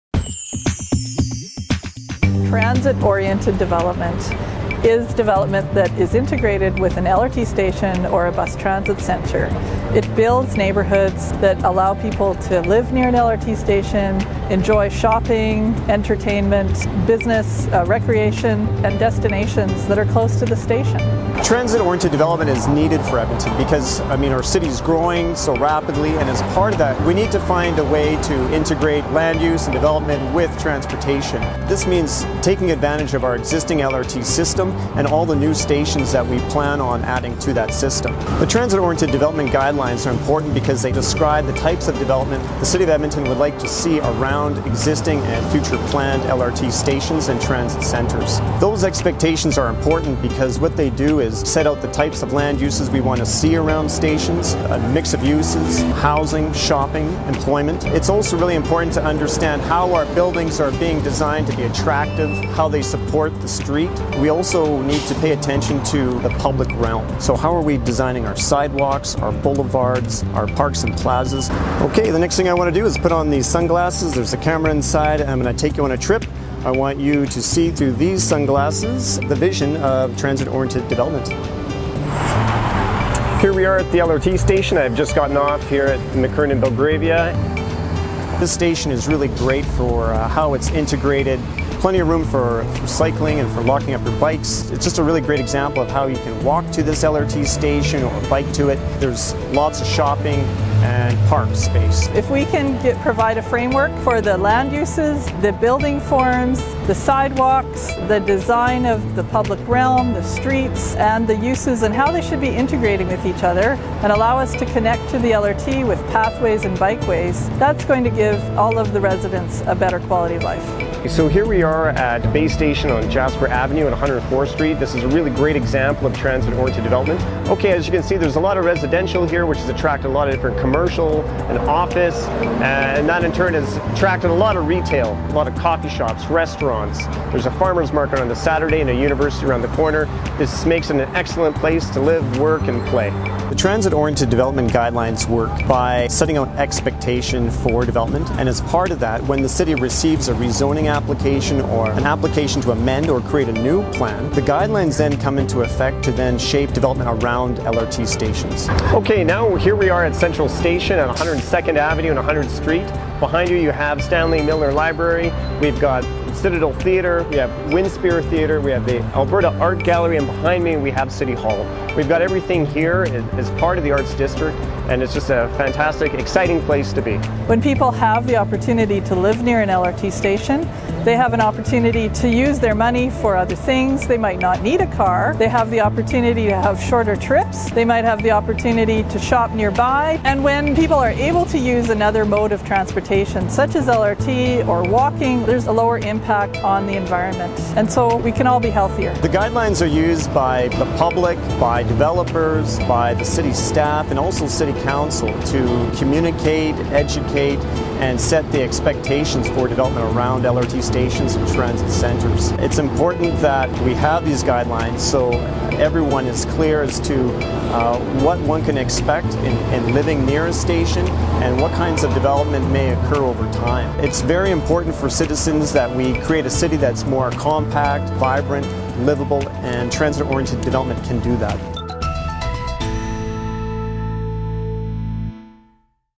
Transit Oriented Development Guidelines
Discover how Edmonton's Transit Oriented Development Guidelines help to guide and integrate development in neighbourhoods around LRT stations and transit centres.